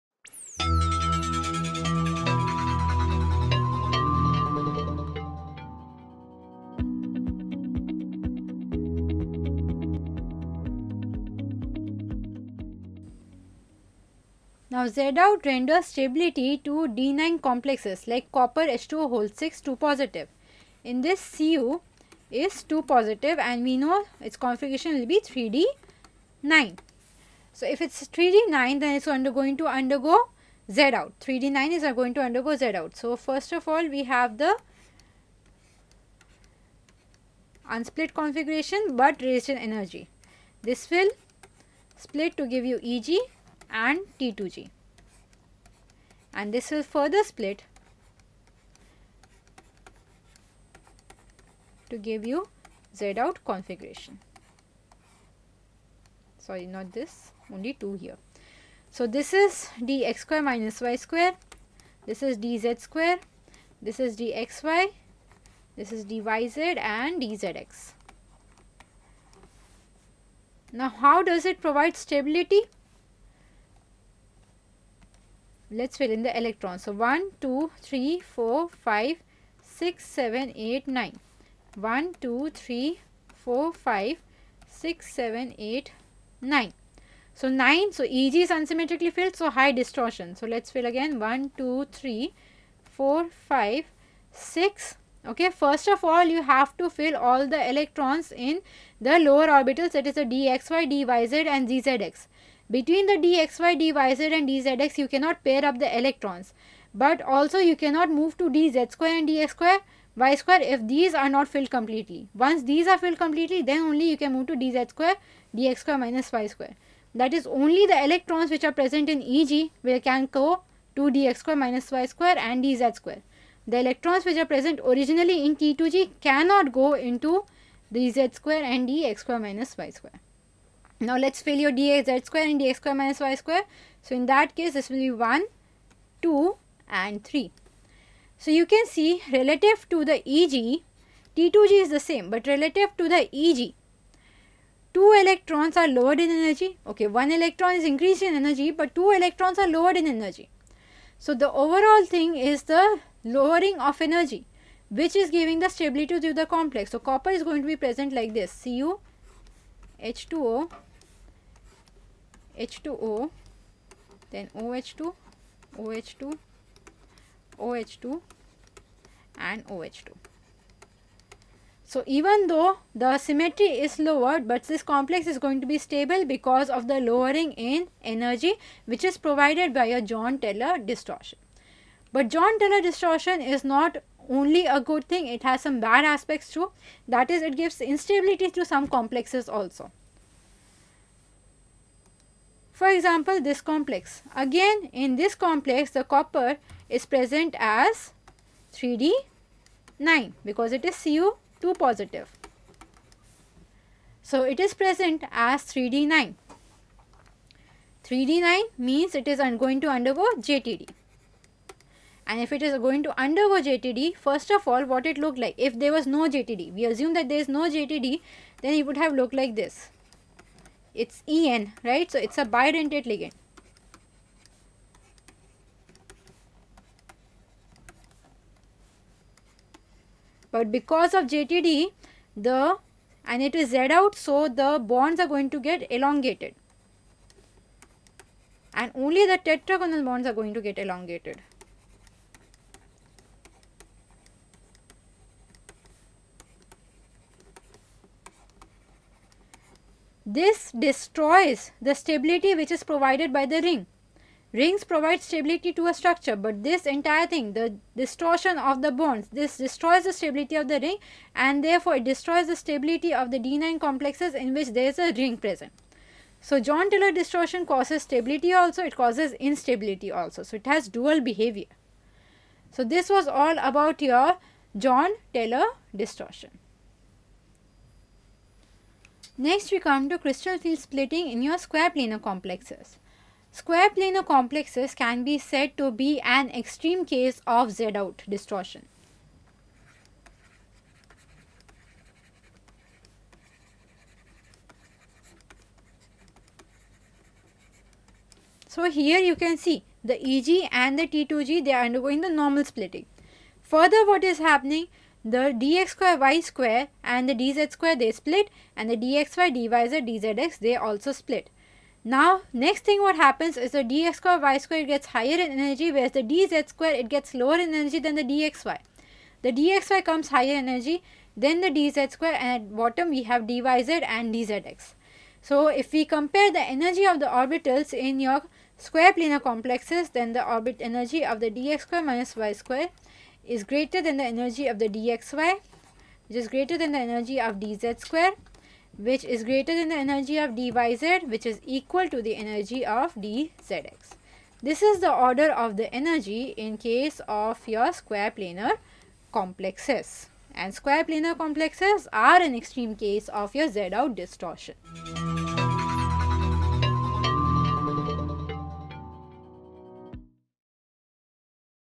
Jahn Teller Distortion | Part 05 | Coordination compounds | JEE | NEET | Chemistry - TG Campus
In this video, we have explained the consequences of the Jahn Teller effect in co-ordination compounds.

Topics covered:
Pairing of Electrons
Questions